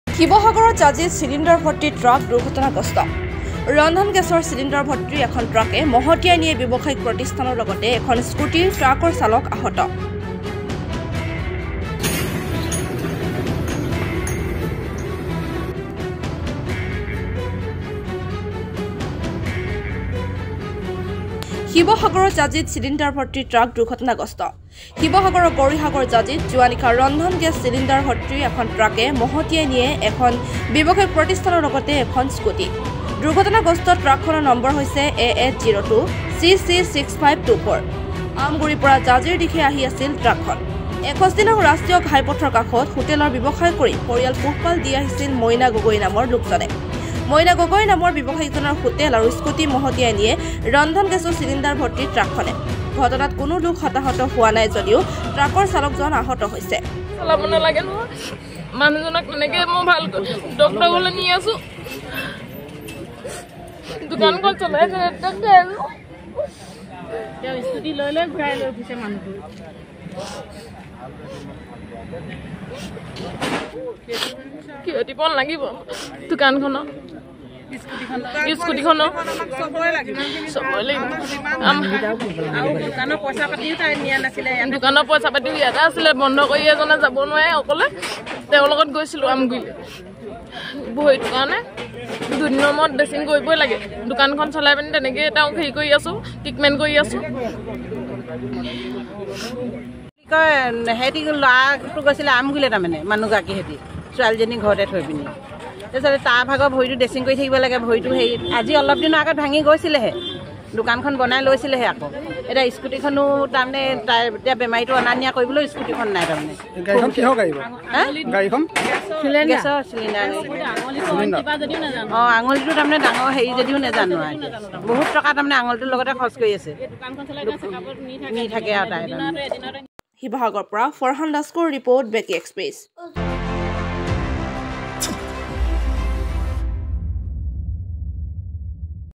ৰন্ধন গেছ চিলিণ্ডাৰ ভৰ্তি এখন ট্ৰাকে মহতীয়াই নিয়ে ব্যৱসায়ীক প্ৰতিষ্ঠানৰ লগতে এখন স্কুটী।
শিৱসাগৰৰ জাঁজীত চিলিণ্ডাৰ ভৰ্তি ট্ৰাক দুৰ্ঘটনাগ্ৰস্ত।